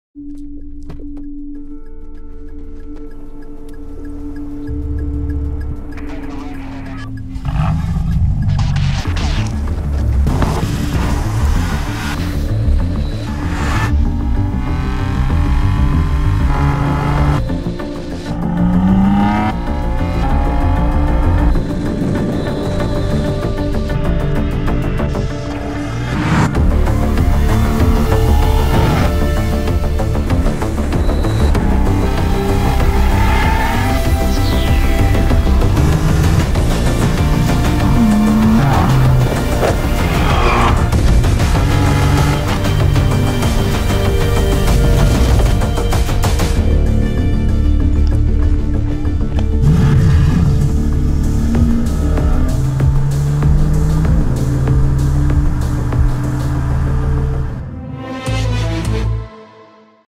2017 BMW M4 CS -- Genuine performance for the racetrack and everyday
The new BMW M4 CS is a special model of the BMW M GmbH in a tradition of unique high-performance automobiles. Sporty dynamics of the highest level, outstanding performance and surprising everyday comfort. The high-performance engine in combination with the aerodynamic lightweight carbon-fibre body propels this extreme sportsman to a Nordschleife lap time of a remarkable 7:38 minutes. Time for a new legend: the BMW M4 CS.
M TwinPower Turbo inline 6-cylinder

Performance 338 kW (460 hp)*
Engine torque (Nm) at 1/min: 600/4,000–5,380*
From 0 to 100 km/h in 3.9 s*